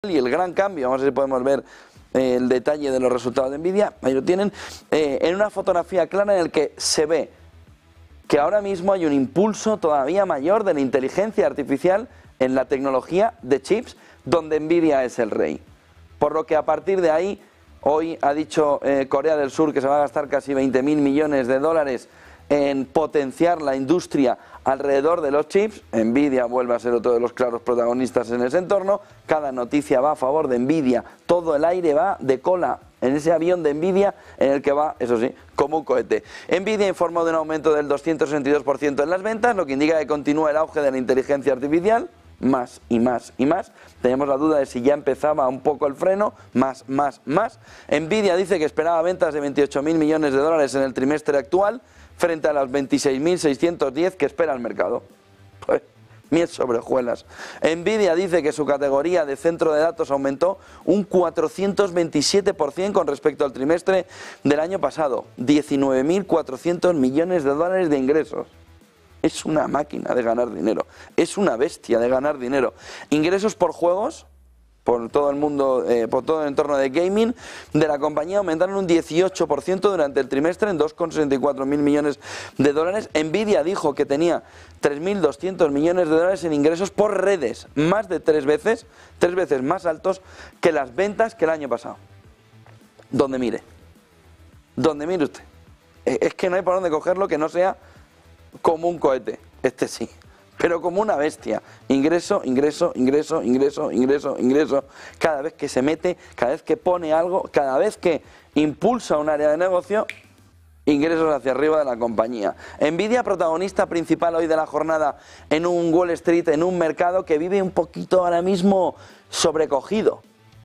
Nvidia, como un cohete: así son sus resultados de la tecnológica
Nvidia ha mejorado los resultados de las expectativas. Esto deja sobre la mesa que la inteligencia artificial está siendo un artífice para el mercado que no para. Está en el entorno de los 1000 dólares de cotización. ¿Qué podemos extraer de los resultados de Nvidia? Hay un impulso de los chips que llevan a Corea del Norte a gastar millones. Y es que, Nvidia ha informado de un aumento del 262% en las ventas, lo que indica que continúa el auge de la IA; se esperaba ventas de 28 mil millones de dólares en el trimestre actual, frente a 26.610 que esperaba el mercado. A su vez, Nvidia dice que su categoría de centro de datos aumentó un 427% con respecto al trimestre del año pasado a 19 mil 400 millones de dólares en ingresos. Estos ingresos por juegos de la compañía aumentaron un 18% durante el trimestre. Por último, Nvidia dijo que tenía 3.200 millones de dólares en ingresos por redes, más de tres veces más altos que las ventas del año pasado.